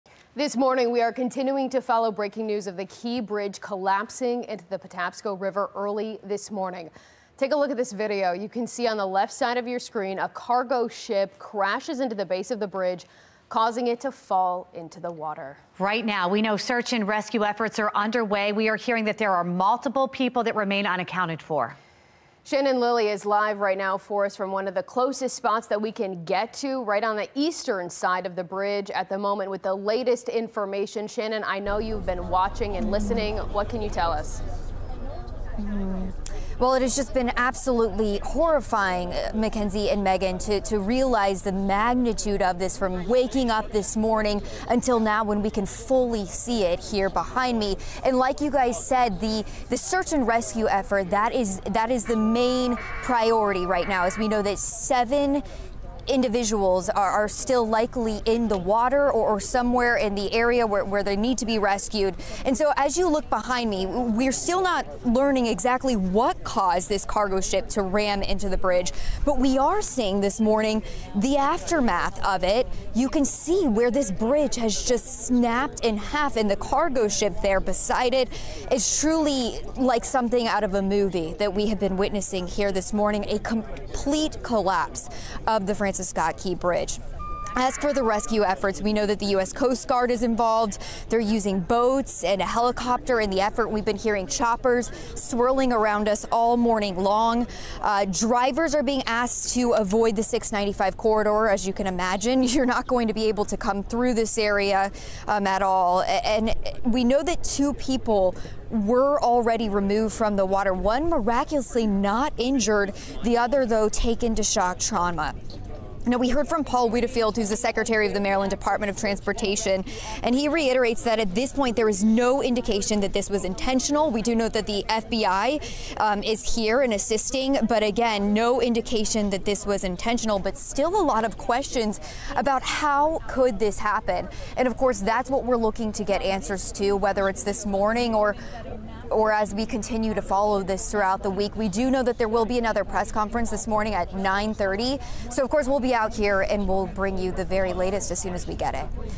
Search and rescue continue after Key Bridge collapses in Baltimore
(WBFF) — The Francis Scott Key Bridge has collapsed into the Patapsco River after a cargo ship collided with it. We heard preliminary calls early this morning around 1:30 a.m.

Chief Kevin Cartwright, director of communications for the Baltimore City Fire Department confirmed that the bridge had been struck by "a large vessel" causing it to collapse into the Patapsco River.